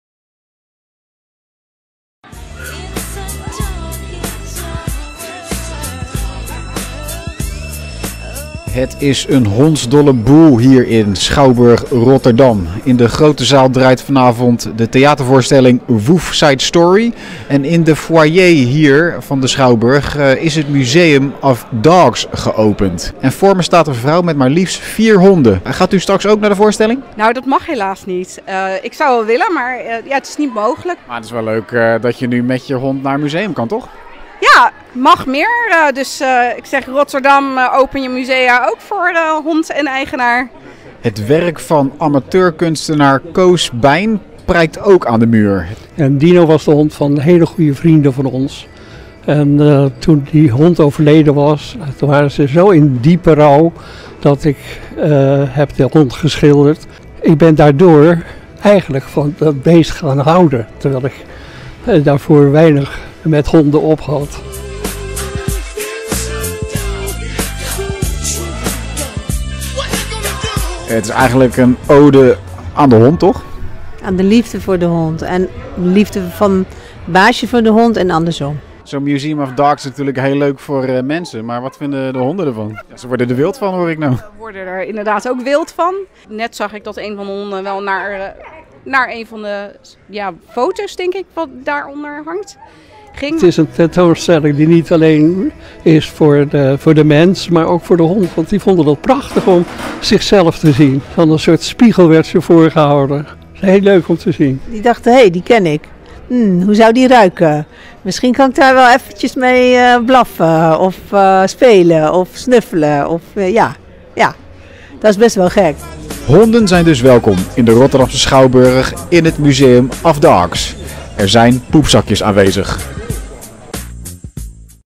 Vrolijke beestenboel in Museum Of Dogs
Het Theater Rotterdam is voor even toegankelijk voor honden. In de grote zaal draait de theatervoorstelling Woef Side Story en in de foyer is het Museum of Dogs geopend.Woef Side Story is ongeveer het verhaal van Romeo en Julia maar dan op z’n hondjes.Het Museum of Dogs toont een muur met allemaal kunstwerken met als thema de beste vriend van de mens. Baasjes en hun honden liepen vrolijk door de Schouwburg.Ode van liefdeOrganisator Mieke van der Linden kijkt vol trots naar de kunstwerken die op de muur prijken. Ze variëren van schilderijen tot keramiek. Voor haar is het een ode van de liefde tussen de hond en zijn baasje.Van der Linden doet veel voor de hondencommunity in Rotterdam. Ze organiseert diverse evenementen voor honden en hun eigenaren zoals de Dogparade. Het idee voor een hondenmuseum is spontaan ontstaan op de fiets en de directeur van de Schouwburg, Melle Damen, vond het meteen een leuk concept.De opening gelijktijdig houden met de theatervoorstelling Woef Side Story  is volgens Van der Linden zeker niet een kat in het bakje. ‘Maar ik had wel iets bij de staart.’KunstHet werk van zelfbenoemd amateurkunstenaar Koos Beijn prijkt ook aan de muur. Het is een schilderij van een mannenlichaam in statige kleding uit de gouden eeuw met een hoofd van een labrador erop geplakt.Hij noemt de hond op het portret Koning Dino en dat is de overleden hond van vrienden van hem. &quot;Dino werd als hun kind opgevoed. Na het overlijden waren die mensen ontroostbaar. Zelf ben ik geen hondenliefhebber maar ik vond het geweldig hoe zij hun hond konden vereren.&quot;Honden toegestaanMuseum of Dogs loopt tot 6 januari. Honden zijn toegestaan en er zijn poepzakjes aanwezig.